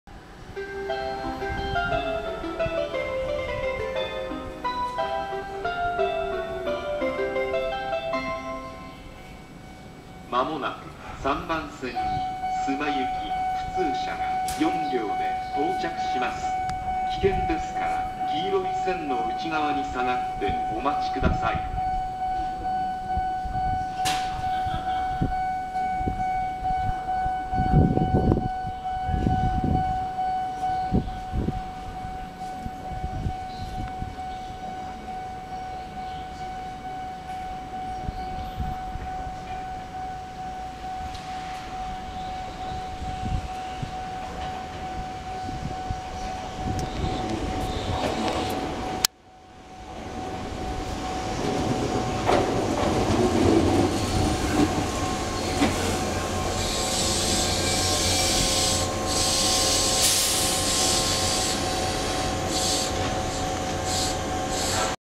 【接近放送入り!】飾磨駅 山陽3000系 普通車須磨行き到着
駅メロも収録してます。
3000系の須磨行き普通車です。